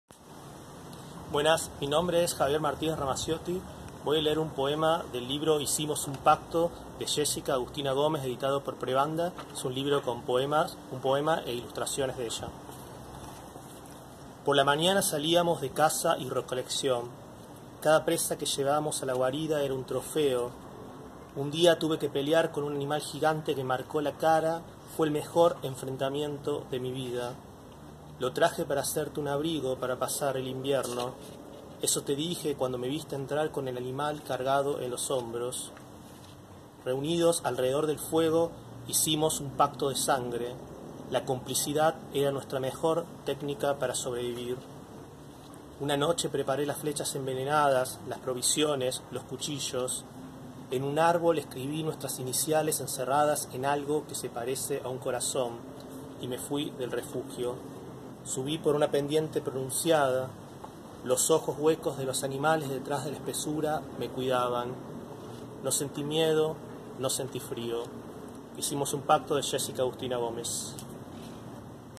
Javier Martínez Ramacciotti lee a Jessica Agustina Gomez
El poeta y editor Javier Martínez Ramacciotti lee Hicimos un pacto, de Jessica Agustina Lopez, editado por Prebanda.

Esta lectura fue grabada en la ciudad de Córdoba durante el período de distanciamiento social obligatorio y compartida en las redes de Poeplas, poesía para las infancias, el 30 de abril de 2020.